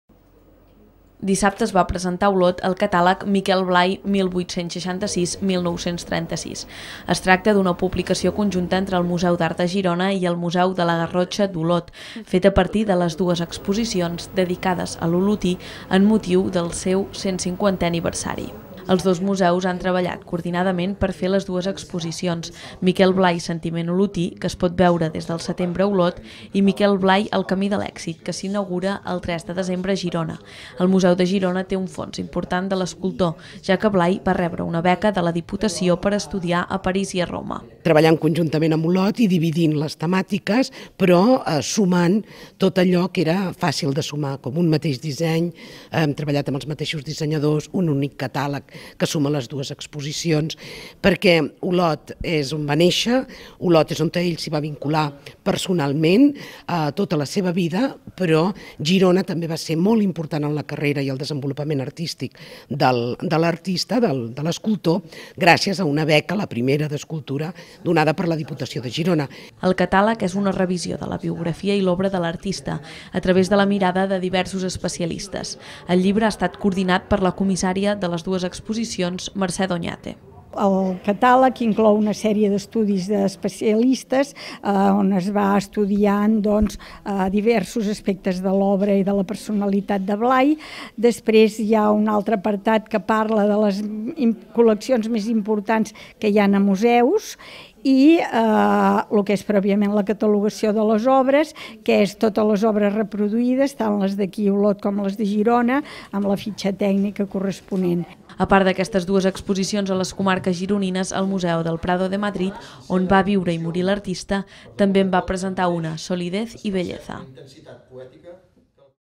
Publiquen el catàleg ‘Miquel Blay 1866-1936’
En el marc de la celebració del 150è aniversari del naixement de Miquel Blay, el Museu de la Garrotxa d’Olot i el Museu d’Art de Girona van presentar dissabte el catàleg ‘Miquel Blay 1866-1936’.